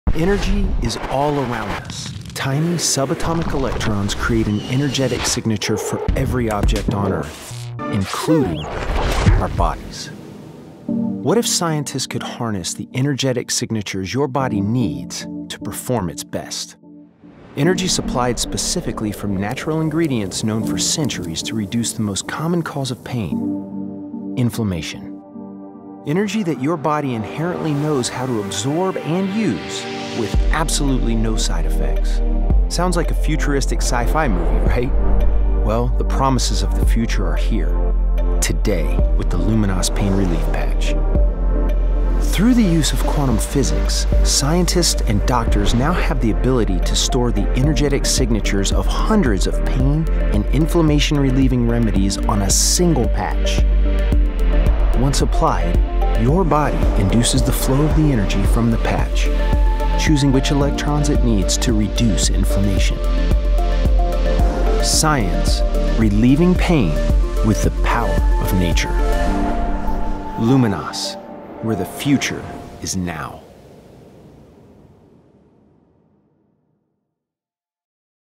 How It Works
Get an in depth look into how the technology behind the Luminas pain relief patch works.